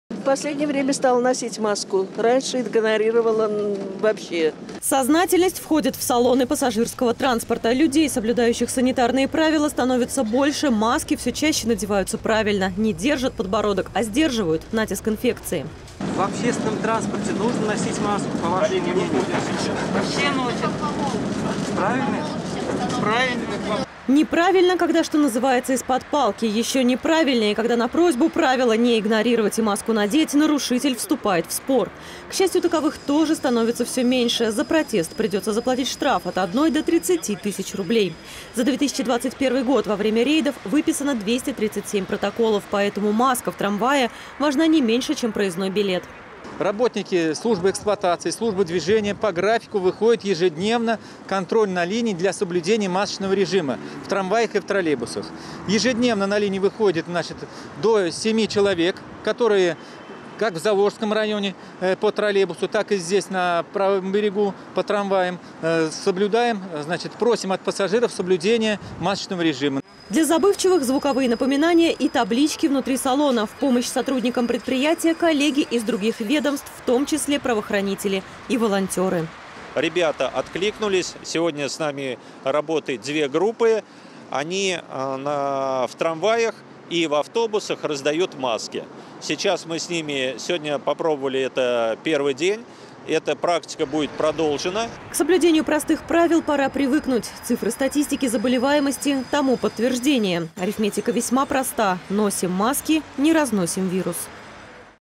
Смотреть сквозь пальцы на нарушителей масочного режима в Ульяновске не будут. Ограничения ужесточены
Усилить контроль. В тесном салоне, забитом под завязку, и даже в полупустом общественном транспорте условия для передачи инфекции самые благоприятные. Помимо ковида, в регионе свирепствуют сезонные простудные заболевания, что качеству медицинской помощи не способствует.

Смотреть сквозь пальцы на нарушителей масочного режима не будут. Ограничения ужесточены. Контроль необходим. И начинать нужно каждому - с себя.⠀⠀⠀⠀⠀